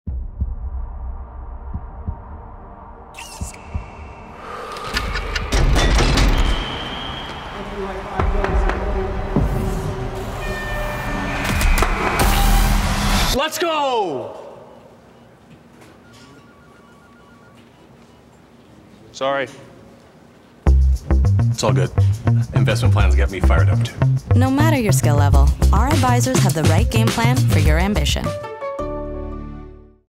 CIBC | Game Plan
On and off the ice, Connor McDavid loves a winning strategy. Talk to an advisor today about a game plan that’s right for your ambition.